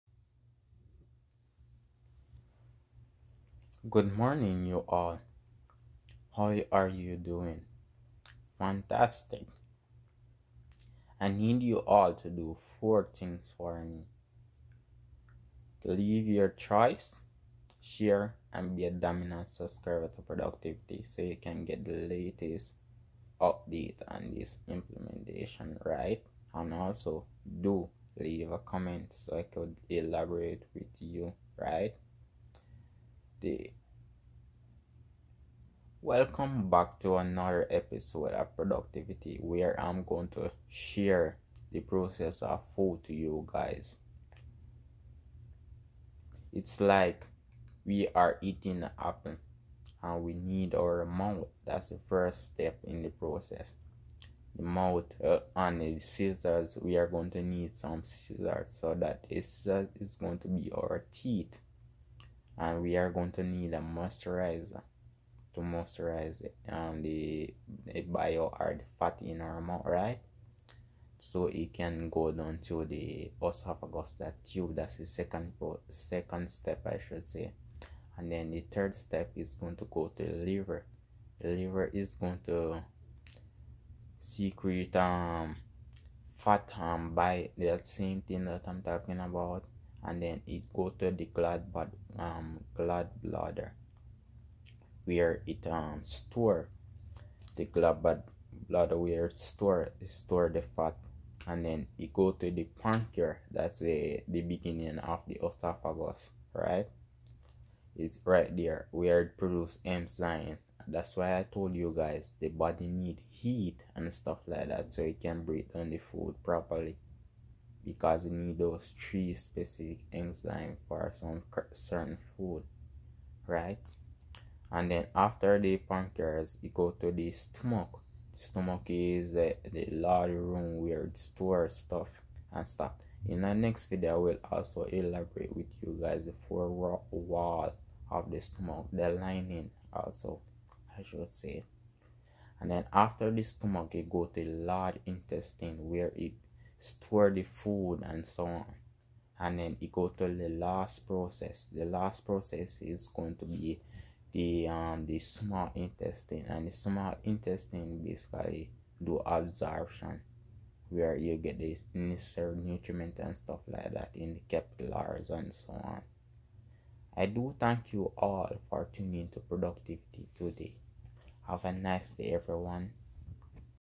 Process of Food
The esophagus can be divided into three parts: cervical, thoracic, and abdominal. Its blood supply, lymphatic drainage, innervation, and architecture of the esophageal wall are described. The topographic relationships of the esophagus and the gastroesophageal junction with neighboring structures are illustrated from the right and left thoroscopic and the laparoscopic viewpoints. Functionally, the esophagus consists of the upper esophageal sphincter; the esophageal body; and the lower esophageal sphincter. Their coordinated muscular activity transports the food bolus into the stomach, while maintaining a barrier against reflux of esophageal contents into the pharynx and gastric juice into the esophagus.